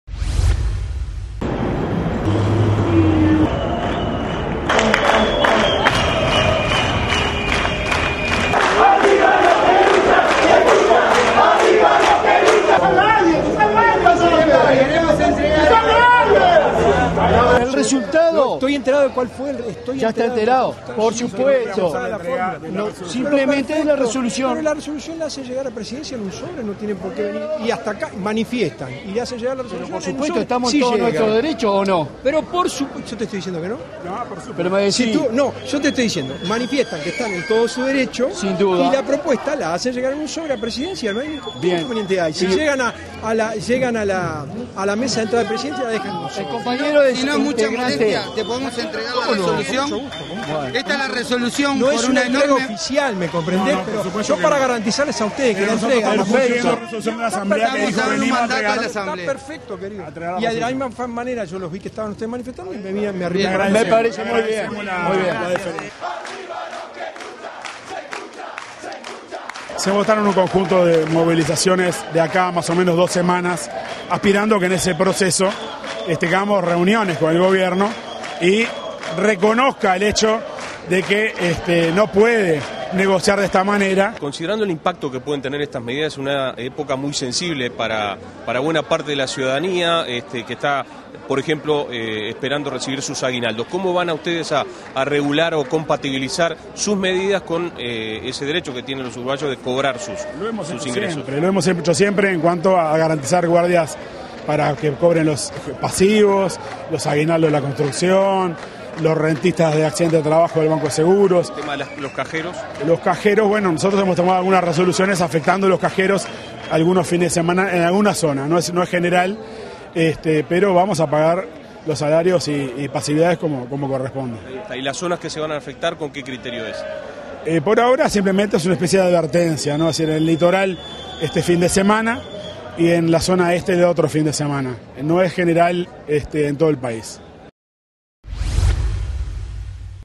Aebu rechazó acuerdo y habrá nuevas medidas de lucha
Los bancarios rechazaron propuesta del gobierno y votaron plan de movilizaciones. Al cierre de la votación hubo incidentes con golpes de puño entre agremiados